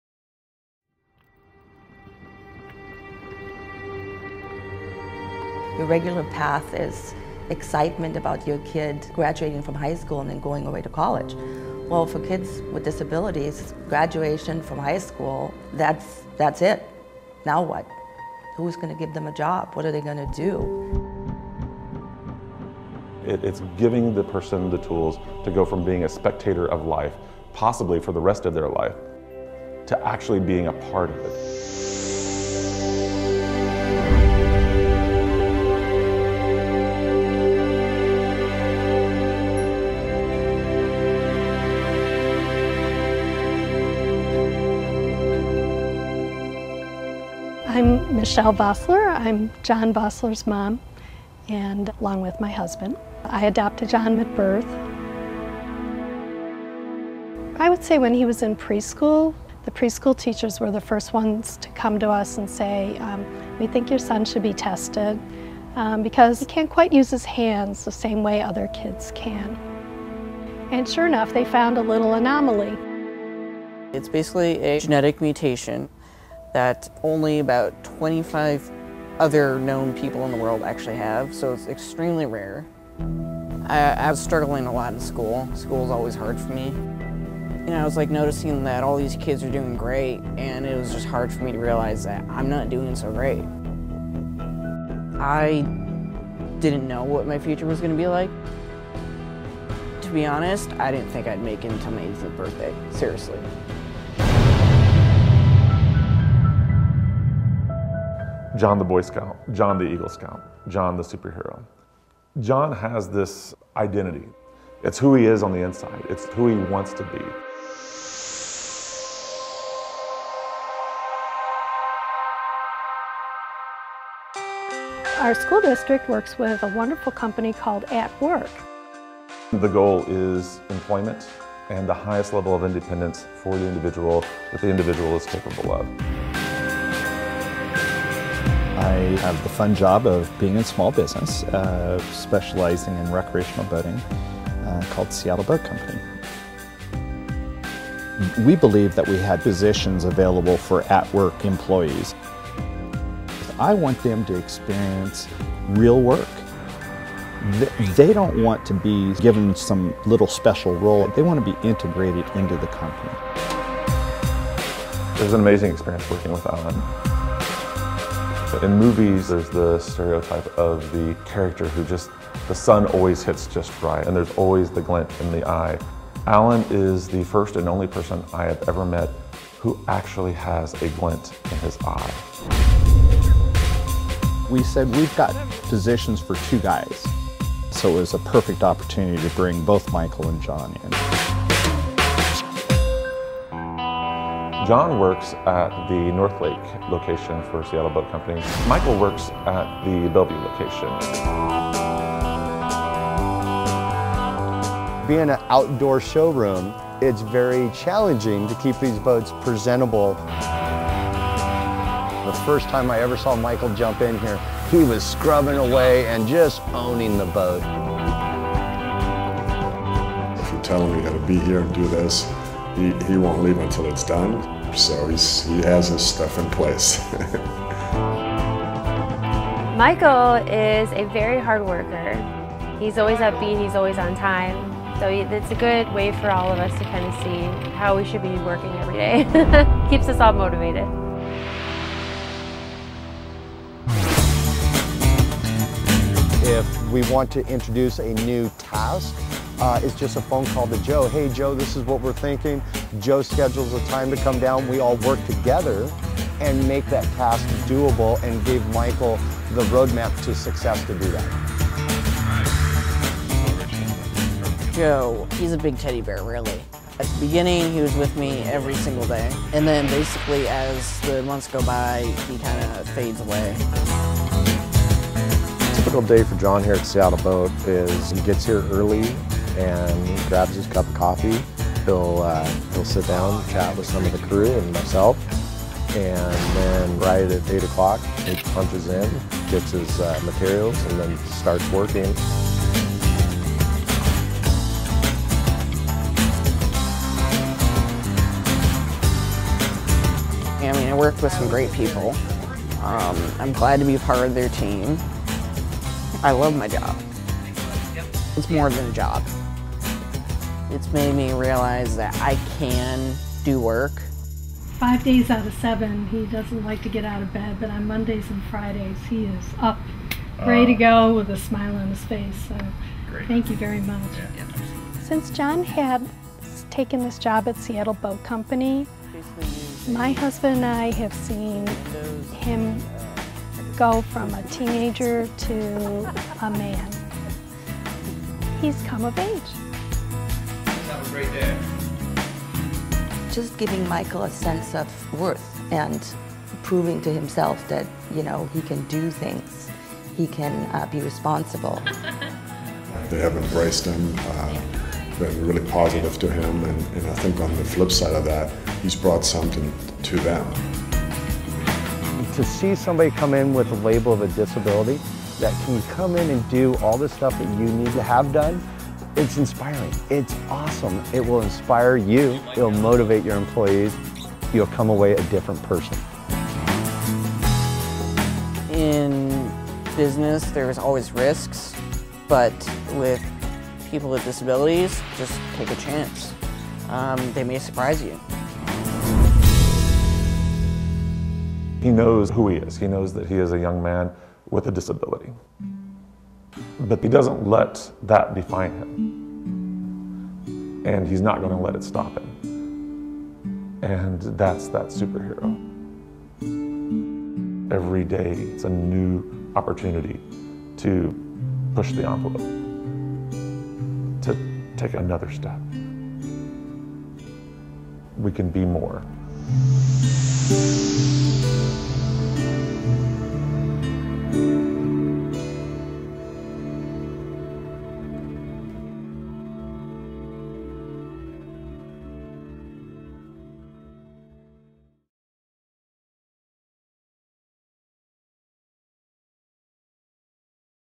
Seattle Boat Company - AtWork! 2014 Supporter of the Year
AtWork! is proud to recognize Seattle Boat Company as our 2014 Supporter of the Year. Learn about this awesome company and the two individuals with disabilities that they employ.